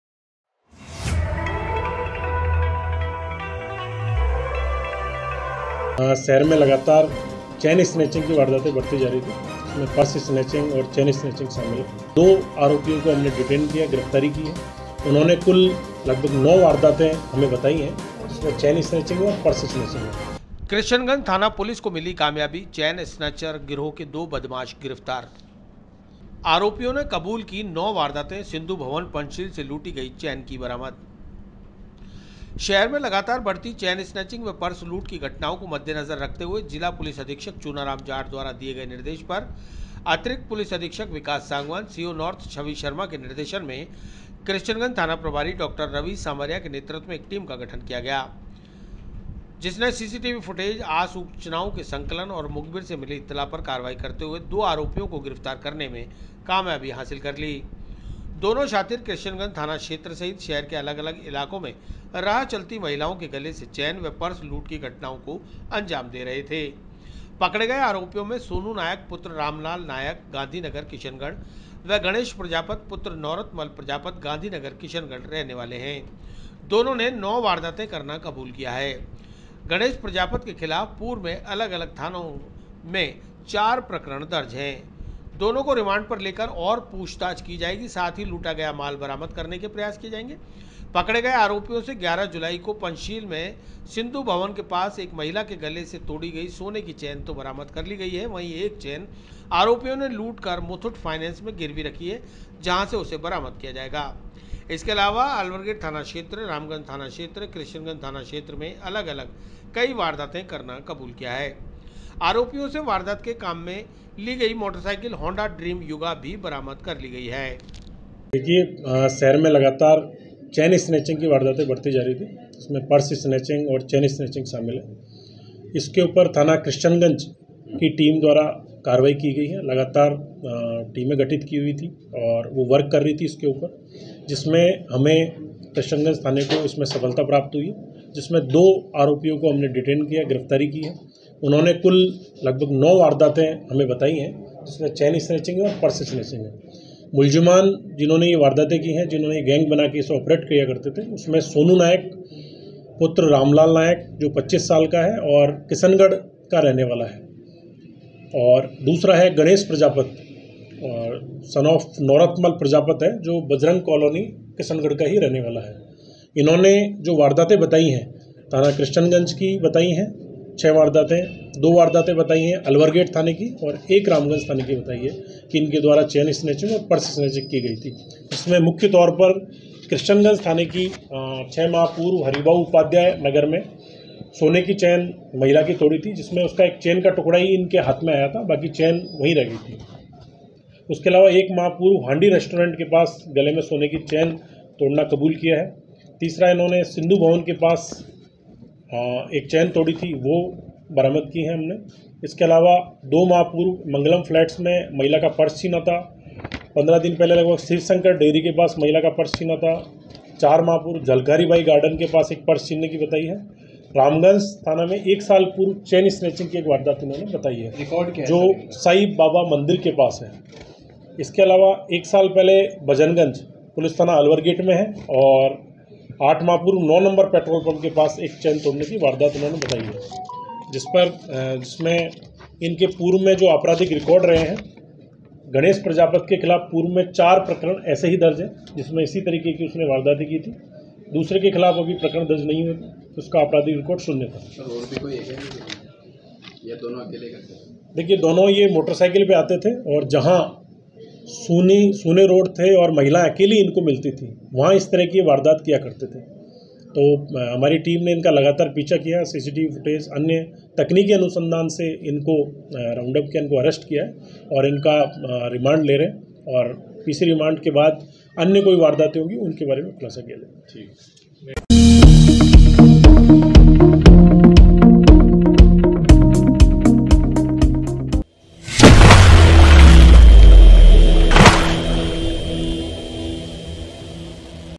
क्रिश्चियन गंज थाना पुलिस को मिली कामयाबी, चेन स्नैचर गिरोह के 2 बदमाश गिरफ्तार
आरोपियों ने कबूल की 9 वारदातें, सिंधु भवन पंचशील से लूटी गई चेन की बरामद

शहर में लगातार बढ़ती चेन स्नेचिंग व पर्स लूट की घटनाओं को मद्देनजर रखते हुए जिला पुलिस अधीक्षक चूनाराम जाट द्वारा दिए गए निर्देश पर अतिरिक्त पुलिस अधीक्षक विकास सांगवान सीओ नार्थ छवी शर्मा के निर्देशन में क्रिश्चियन गंज थाना प्रभारी डॉ रविश सामरिया के नेतृत्व में एक टीम का गठन किया गया। जिसमें सीसीटीवी फुटेज , आ सूचनाओं के संकलन और मुखबिर से मिली इत्तला पर कार्यवाही करते हुए दो आरोपियों को गिरफ्तार करने में कामयाबी प्राप्त कर ली है। दोनों शातिर क्रिश्चियन गंज थाना क्षेत्र सहित शहर के अलग-अलग इलाकों में राह चलती महिलाओं के गले से चेन व पर्स लूट की घटनाओं को अंजाम दे रहे थे। पकड़े गए आरोपियों में सोनू नायक पुत्र रामलाल नायक गांधी नगर किशनगढ़ व गणेश प्रजापत पुत्र नोरत मल प्रजापत गांधी नगर किशनगढ़ के रहने वाले हैं। दोनों ने 9 वारदातें करना कबूल किया है। गणेश प्रजापत के खिलाफ पूर्व में अलग-अलग थाना क्षेत्रों में 4 प्रकरण दर्ज है। दोनों को रिमांड पर लेकर और पूछताछ की जाएगी साथ ही लूटा गया माल बरामद करने के प्रयास किए जाएंगे। पकड़े गए आरोपियों से 11 जुलाई को पंचशील में सिंधु भवन के पास एक महिला के गले से तोड़ी गई सोने की चेन बरामद की गई है। वही एक चैन आरोपियों ने लूट कर मुथूट फाइनेंस में गिरवी रखी है जहां से उसे बरामद किया जाएगा। इसके अलावा अलवर गेट थाना क्षेत्र, रामगंज थाना क्षेत्र, क्रिश्चियन गंज थाना क्षेत्र में अलग-अलग कई वारदातें करना कबूल किया है। आरोपियों से वारदात के काम में ली गई मोटरसाइकिल हौंडा ड्रीम युगा भी बरामद की गई है।